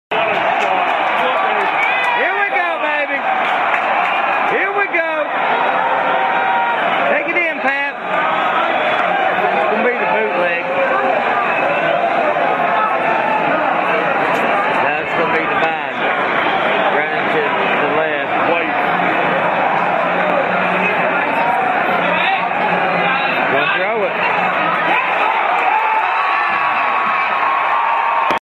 Pat White TD Pass2
Pat White to Dorrell Jalloh 2 yard TD pass.WVU rolls Auburn 34 to 17.